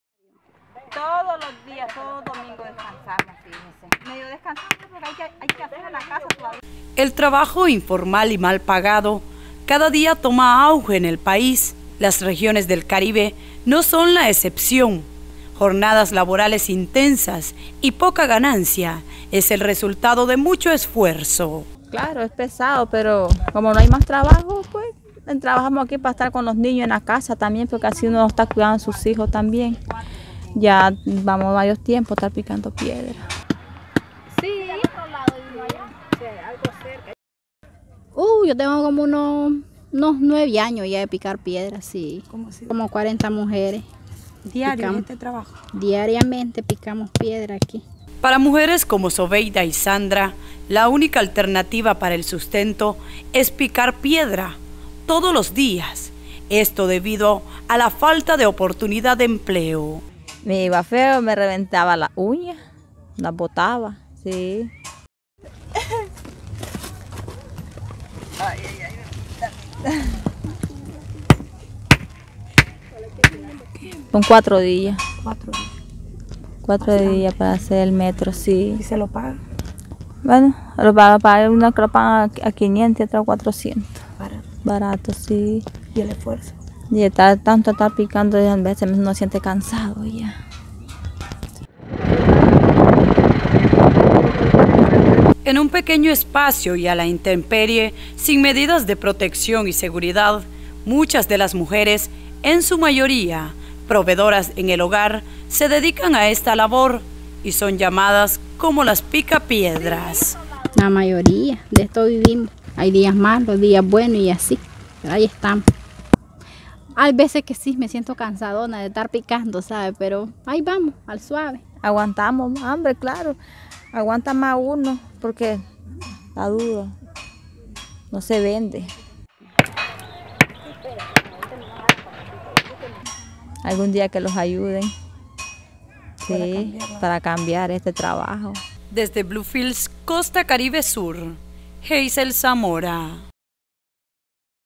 Las mujeres “picapiedras” del Caribe
En Bluefields se les conoce como las “picapiedras” y laboran en condiciones estresantes y deplorables. Un trabajo informal al que cada día se suman más mujeres ante la falta de mejores fuentes de empleo en el caribe.

Sentadas sobre la tierra y a la intemperie, sin medidas de protección físicas ni sanitarias, muchas de las mujeres - en su mayoría madres solteras- se dedican a esta labor, mientras sus hijos rondan entre las piedras ya pulverizadas o a veces se suman a la dura faena para ayudarlas.

Su piel muestra los estragos de la dura jornada. Su cutis está reseco y enrojecido por el sol. Sus manos agrietadas ya se acostumbraron al golpe incesante del martillo.